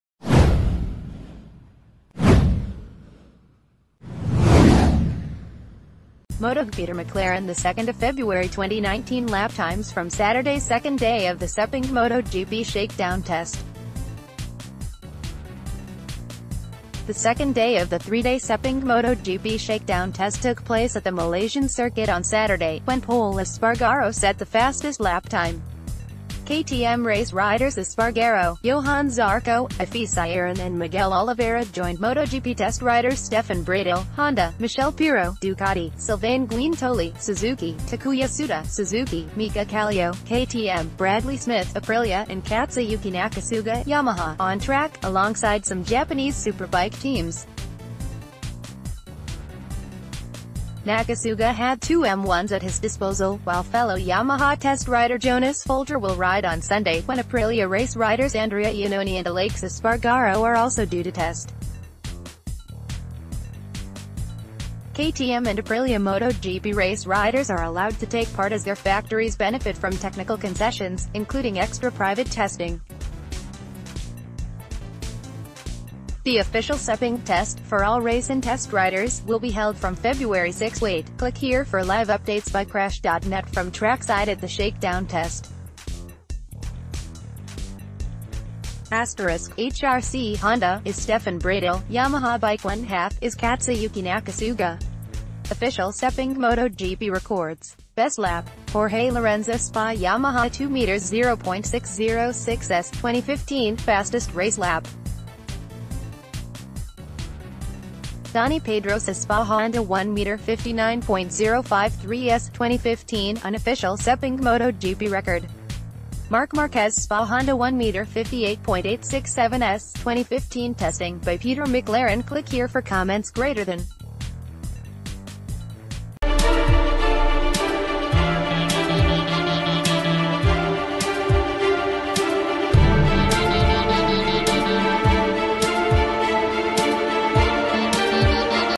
Sepang MotoGP Shakedown test times - Day 2 (FINAL)
MotoGPPeter McLaren2 Feb 2019Lap times from Saturday's second day of the Sepang MotoGP Shakedown test. The second day of the three-day Sepang MotoGP Shakedown test took place at the Malaysian circuit on Saturday, when Pol Espargaro set the fastest lap time. KTM race riders Espargaro, Johann Zarco, Hafizh Syahrin and Miguel Oliveira joined MotoGP test riders Stefan Bradl (Honda), Michele Pirro (Ducati), Sylvain Guintoli (Suzuki), Takuya Tsuda (Suzuki), Mika Kallio (KTM), Bradley Smith (Aprilia) a...